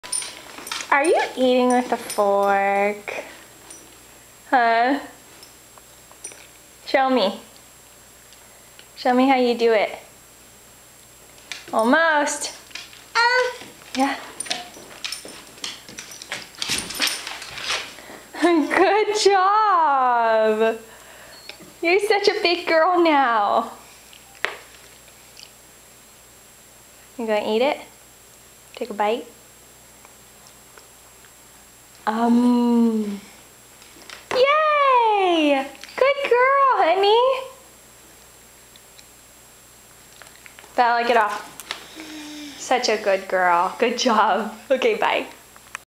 eating with utensils for the first time
rylie (16 months) learns how to use a fork.